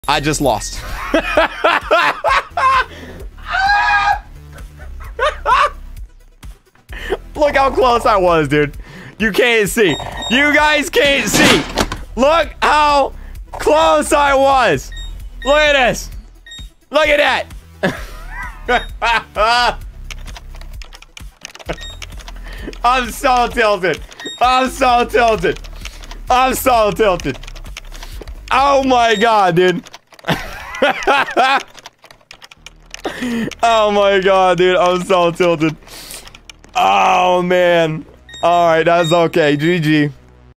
KreekCraft lost $10,000 on LIVE STREAM!! He got tilted 😡
I want cry, cry 😢... KreekCraft wanted to get money but he didn’t, however he did get $700 from donations in the stream lol!